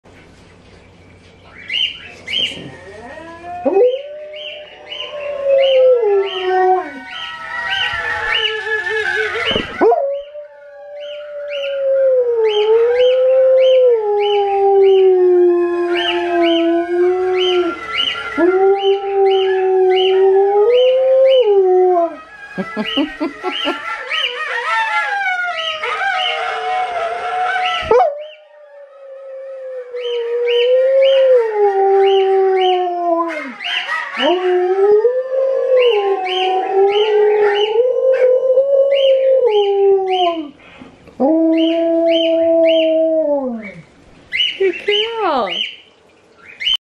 Alaskan Malamutes plotting to take over the world
Alaskan Malamutes are so clever they use modern technology to communicate just like humans. Our Alaskan Malamute  Sojou answered her relatives  Ejo & Niji and it made complete sense to her. Are they plotting to take over the world?